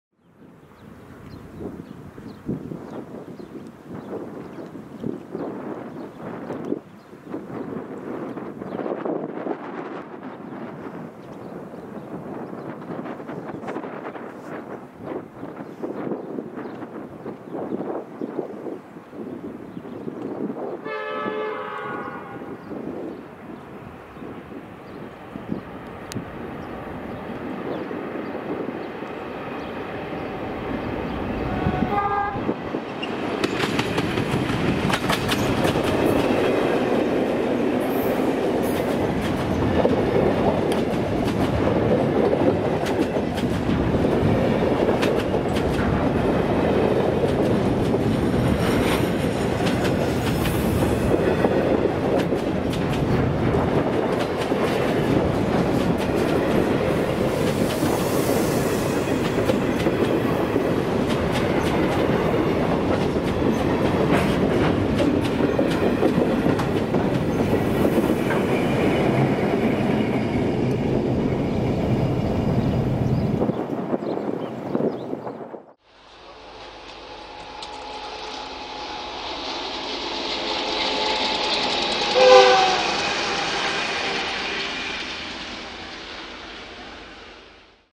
SSR Wagon Transfers Bendigo - 10th Oct 2021
T381-T386+Spoil+Grain+Ballast Wagons+T363 to North Bendigo Workshops
T386-T386+Grain Wagon on Up through Kangaroo Flat (Railcam Footage)